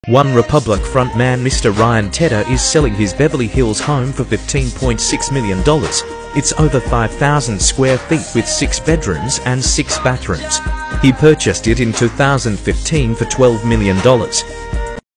Ryan Tedder $15,650,000 Home! 😍
Although best known as the lead singer of the pop band OneRepublic, Ryan Tedder is a longtime singer/songwriter and producer